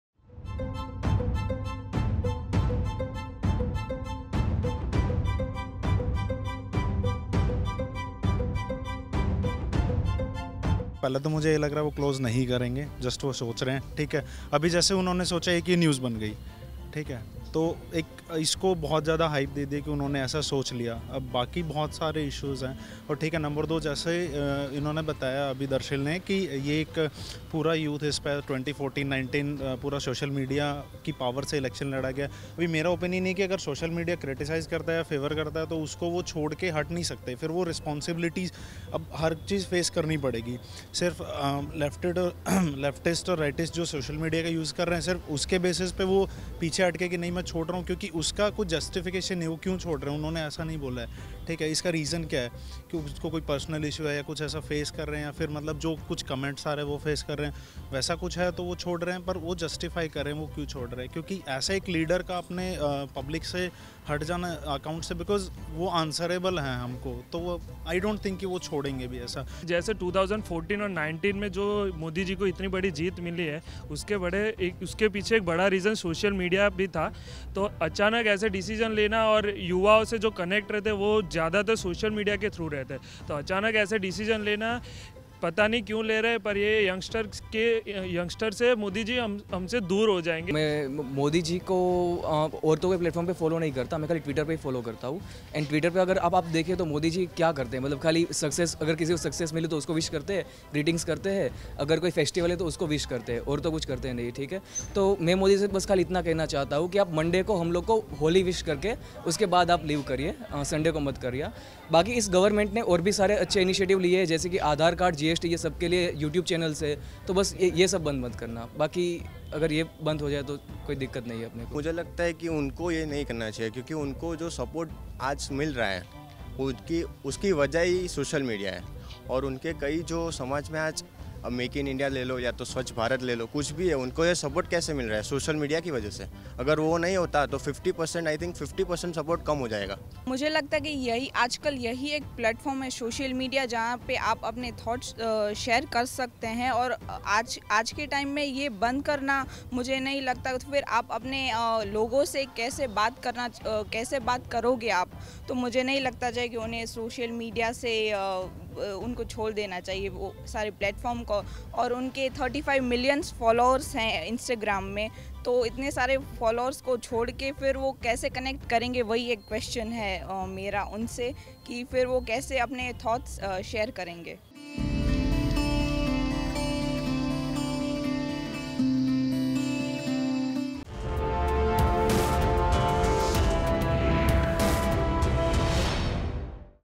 PM Narendra Modi के Social Media छोड़ने पर क्या होता | Gujarat Tak
Youth Review on PM Modi's Social Media quit Tweet